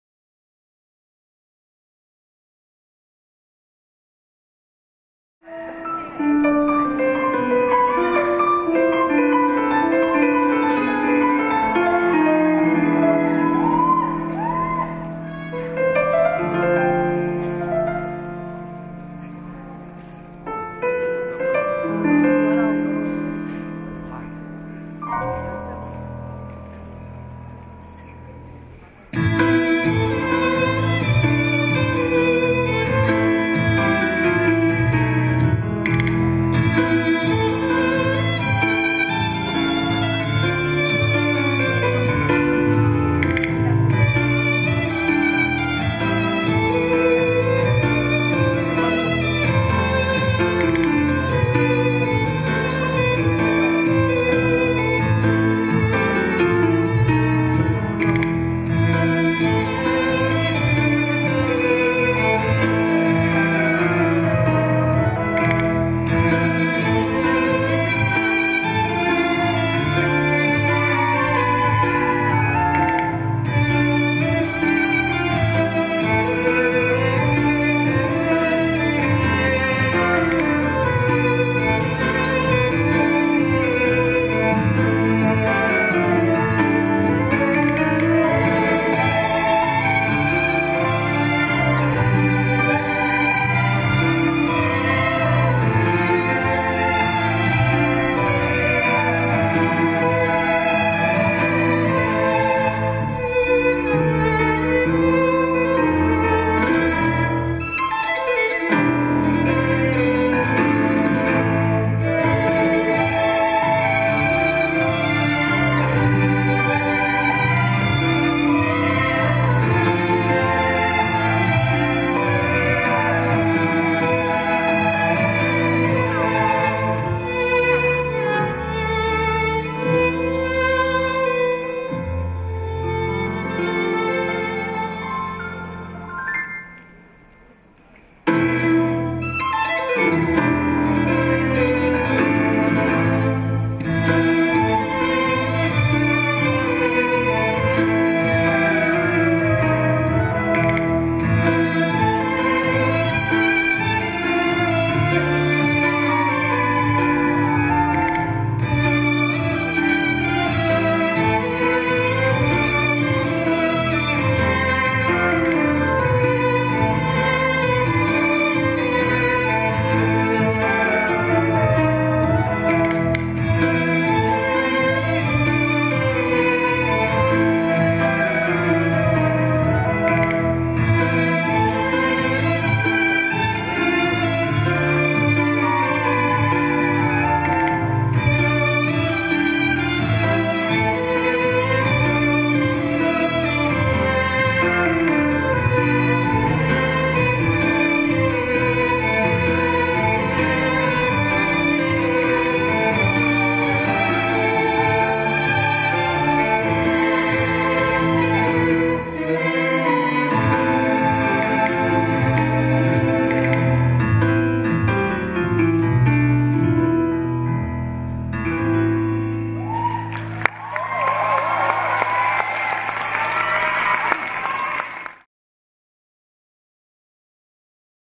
서울종합예술학교 무용예술학부 정기발표회 재즈댄스 'Violet Tango'
Mnet 댄싱9 플러스에디션 심사위원 KiKi 서수지교수님 지도아래~ 전국무용경연대회 교육부 장관상을 수상한 최정렬학생이 출연하여 선보이는 재즈댄스 정기발표회~!

이상형, 이재은, 김소미, 이수진학생들과 함께 실연의 비운을 뒤로한 마음을 온몸으로 표현합니다~!

함께 감상하세요~!